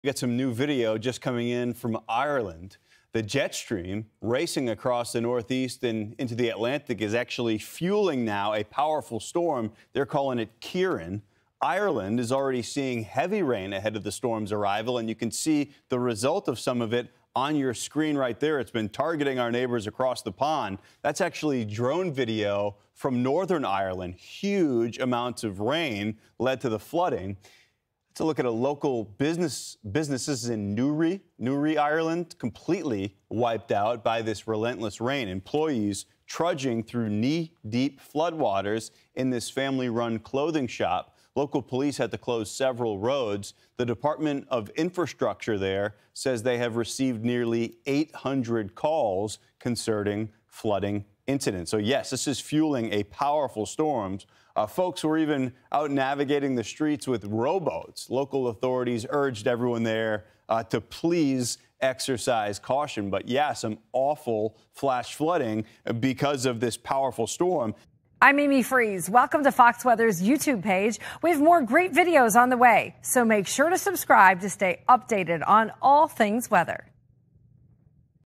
Drone Video Shows Northern Ireland Town Underwater From Storm Days Before Ciaran Strikes
Newry, Ireland sits underwater as drone video shows widespread flooding ahead of Storm Ciaran.